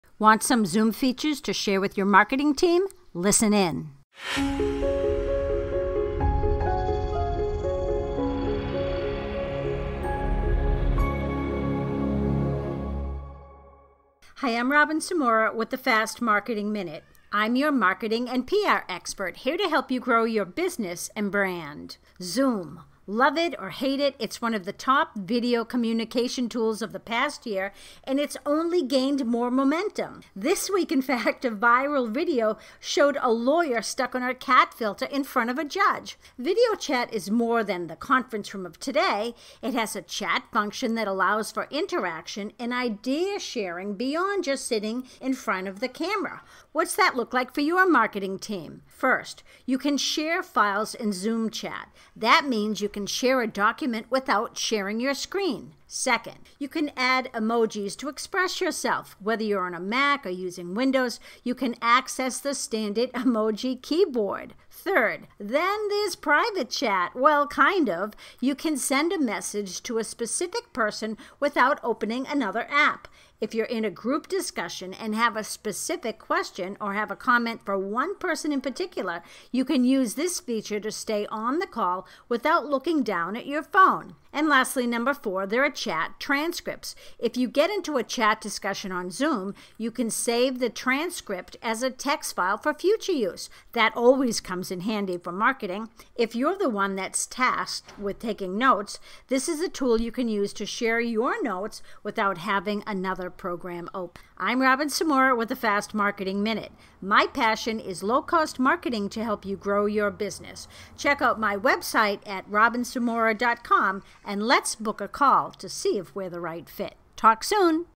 Zoom Features for Your Marketing Team
Want some Zoom features to share with your marketing team? Listen in.

Zoom. Love it or hate it, it is one of the top video communication tools of the past year and it’s only gained more momentum. This week, a viral video showed a lawyer stuck on a cat filter in front of a judge.

Video chat is more than the conference room of today. It has a chat function that allows for interaction and idea sharing beyond just sitting in front of the camera. What’s that look like for your marketing team

1. Share files in Zoom chat. That means you can share a document without sharing your screen.

2. Add emojis to express yourself. Whether you’re on a Mac or using Windows, you can access the standard emoji keyboard.

3. Then there’s private chat. Well sort of. You can send a message to a specific person without opening another app. If you’re in a group discussion and have a specific question or have a comment for a one person in particular, you can use this feature to stay on the call without looking down at your phone.

4. Chat transcripts. If you get into a chat discussion on Zoom, you can save the transcript as a text file for future use for marketing. If you’re tasked with taking notes, this is a tool you can use to share your notes without having another program open.

I’m Robin Samora with the Fast Marketing Minute. My passion is low-cost marketing to help you grow your business. Talk soon!

The post Zoom Features for Your Marketing Team appeared first on Fast Marketing Minute .